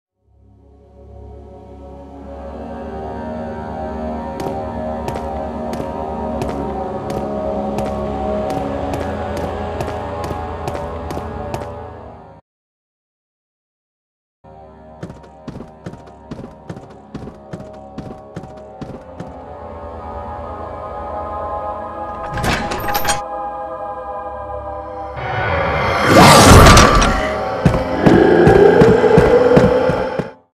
Amnesia - In Real Life: A Felejtés Szigete (próba1)
Ez 1 próba videó, ahol az effekteket próbálgattam és szerintem egész jó lett... :D